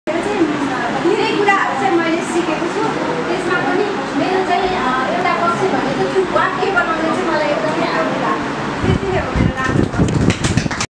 Training
Micro teaching practice by staff during the capacity building training of CDS